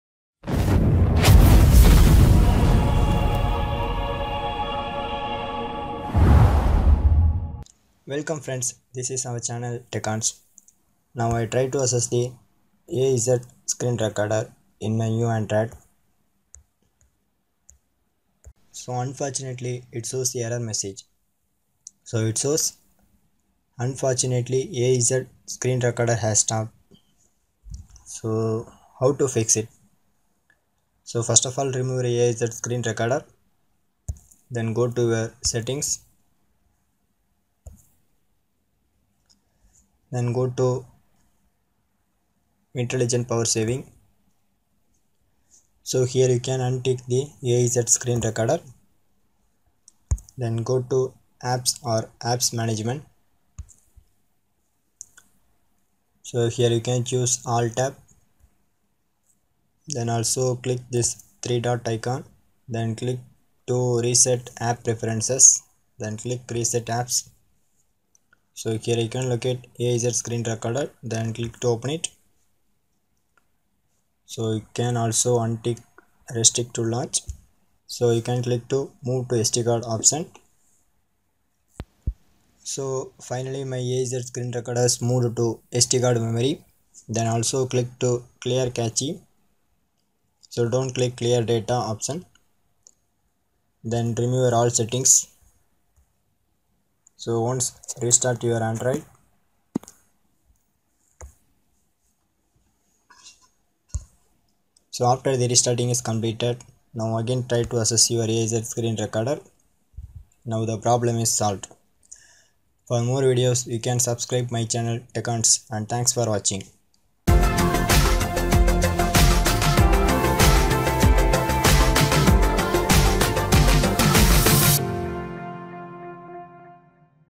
Fix Unfortunately AZ Screen Recorder has stopped working in Android|Tablet
Fix Unfortunately AZ Screen Recorder has stopped working in Android|Tablet-az screen recorder stop recording-unfortunately az screen recorder has stopped.

I get the following error message/notification while try to access AZ screen recorder in my android:

Unfortunately,AZ Screen Recorder has stopped.

ok       Report

This error solution is common for android,tablet,bluestacks and the android operating systems are kitkat,lollipop,marshmallow and nougat also.

Solution:

1.Goto your settings - intelligent power savings, here you can untick "az screen recorder".

2.Then goto apps or application management, here you can choose " all " tab.

3.Also click this three dot icon and click to "reset app preferences" option.

4.Then locate " az screen recorder " and click to open it.

5.Here you can click to " move to sd card " option and also click " clear cache" option.

6.once restart/reboot your android mobile, finally the problem is solved.